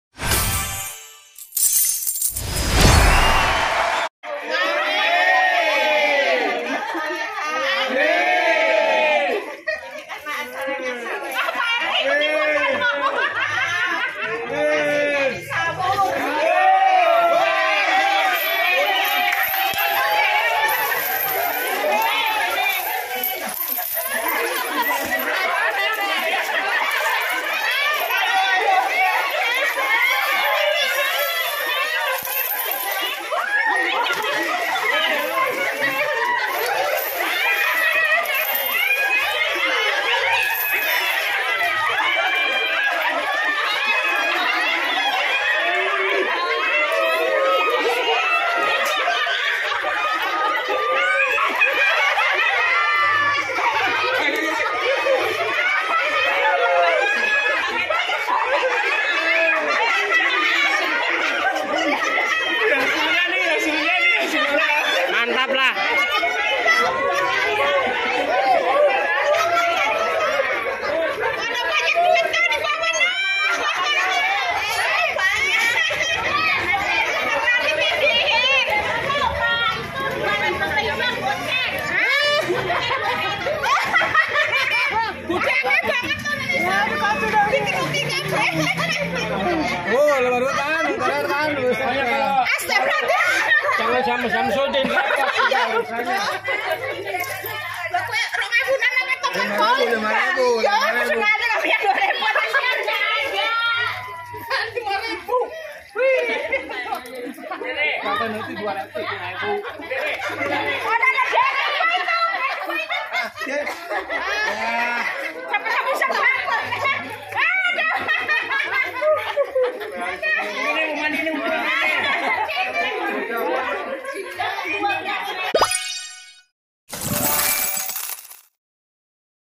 Saweran menang banyak ,, Receh tapi seru dan lucu
Selamat sore.
Beginilah suasana di komplek kalau lagi ada yang berbagi rejeki dengan cara sawer menyawer karena anak balita dari salah satu warga ada yang mulai bisa berjalan.
Tak peduli laki-laki, perempuan, tua, muda dan anak-anak akan senantiasa antusias untuk menyambut dengan gembira, tak peduli apapun yang disawerkan dan berapapun jumlah uang yang disawerkan.